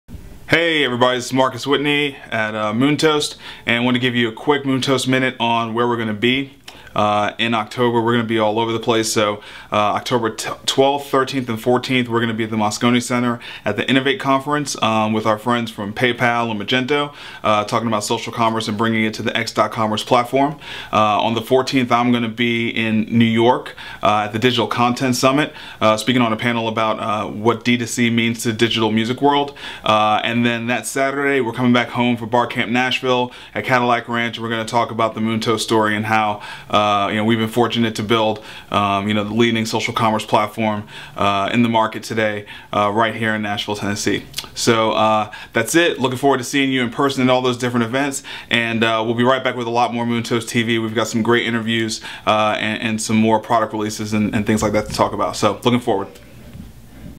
Moontoast Minute Episode 4 - Moontoast is everywhere.
Moontoast is hitting a bunch of upcoming conferences. Wil you be there? Marcus gives you the run-down.